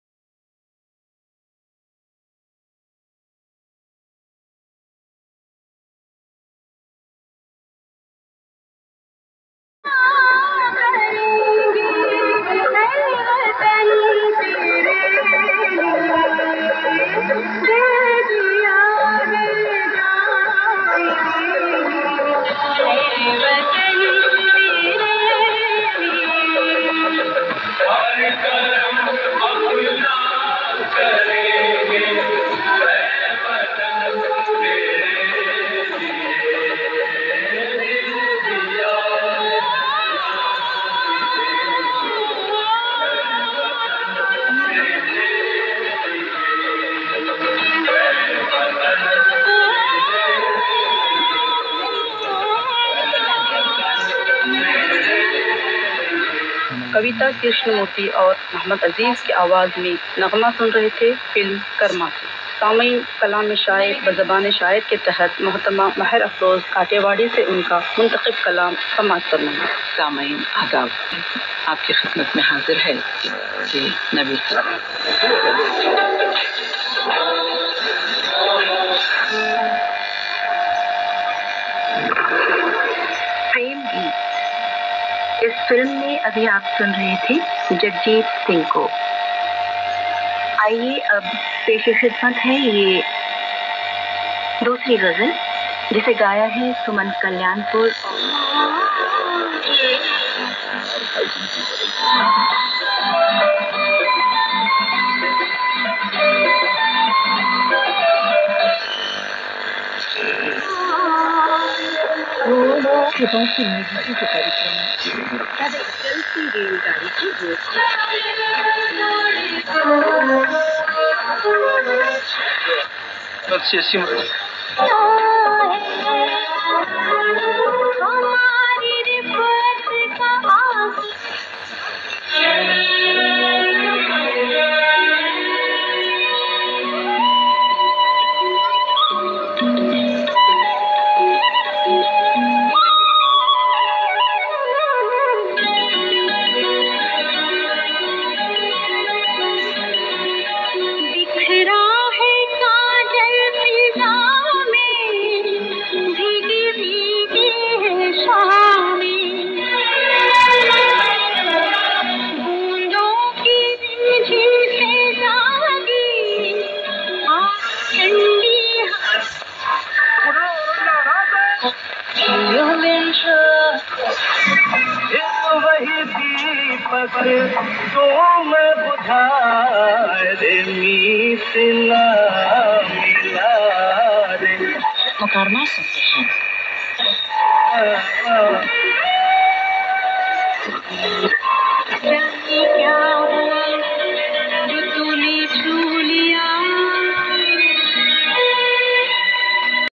Philips prince Deluxe radio tuning.founding many more station.all full working.transister radio
Philips vintage transister radio
Philips prince Deluxe radio